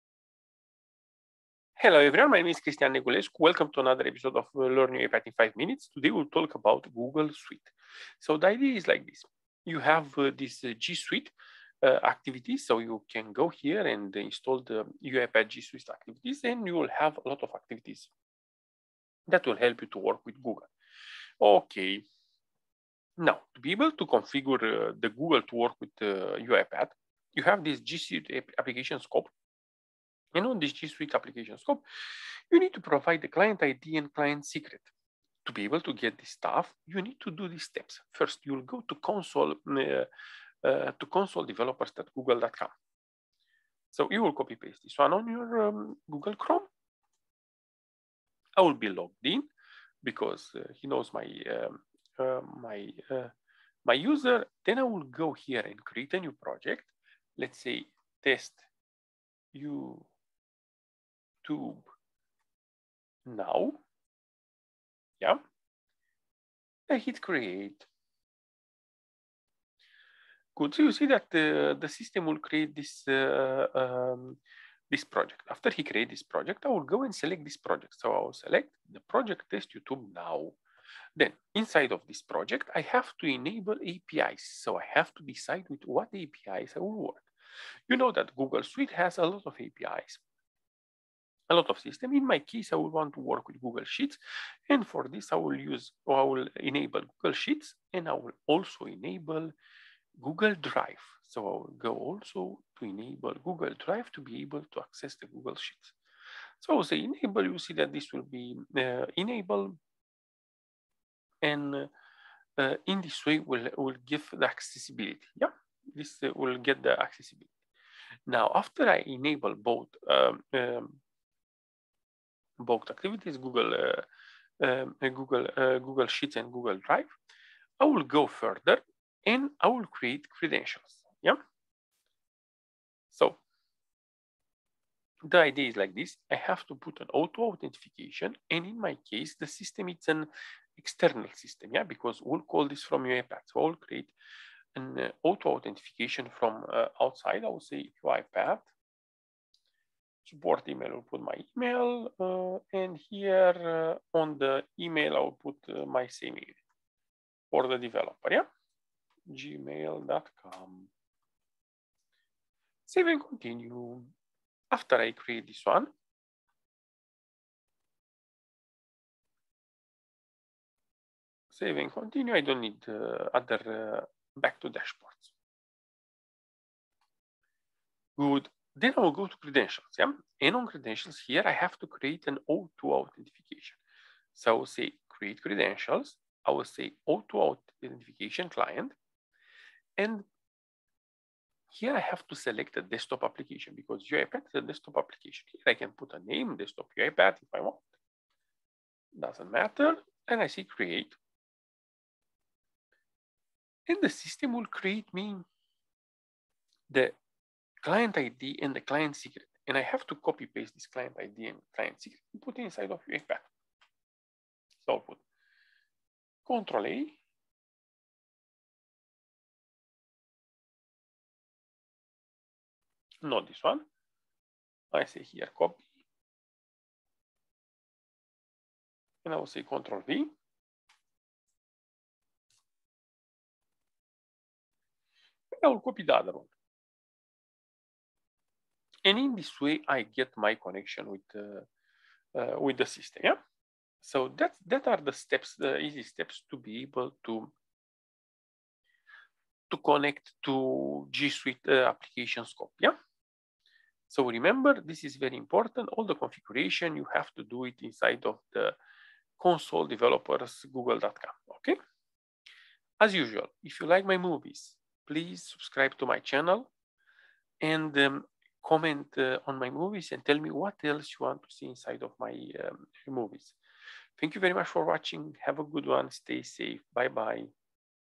How to Set Up GSuite for UiPath Studio
GSuite configuration to work with UiPath Studio | UiPath in 5 minutes | Ep:34